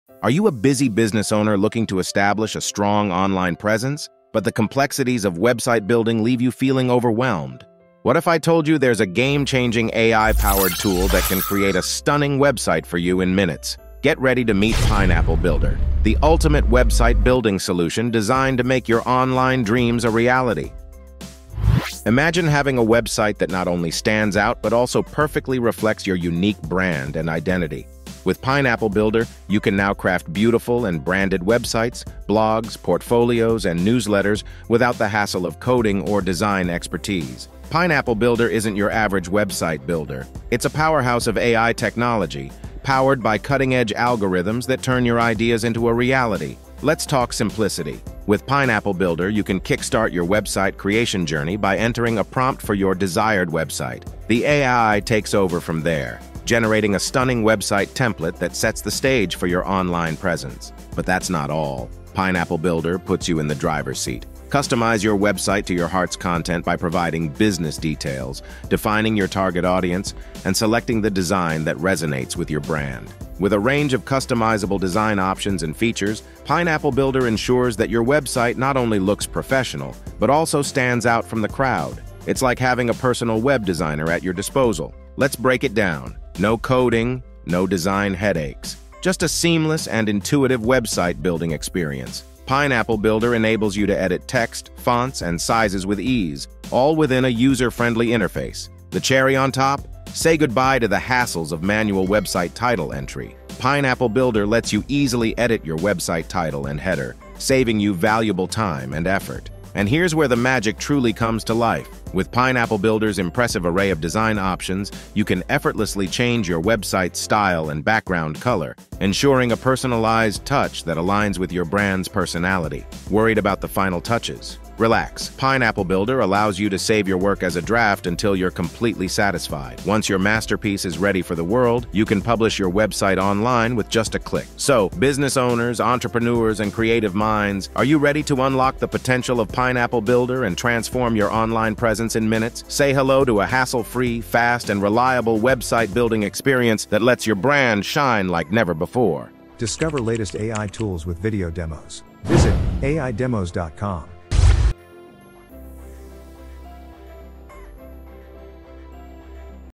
Create a Website in Seconds with Pineapple Builder's AI Website Builder | Pineapple Builder Demo
In this AI Demo, we'll be showcasing Pineapple Builder, a revolutionary platform that allows busy business owners to create a website in just seconds using artificial intelligence.

Using Pineapple Builder, you can quickly build a website with a professional look and feel, no coding required. It also includes tools to help you optimize your website performance and maximize online visibility.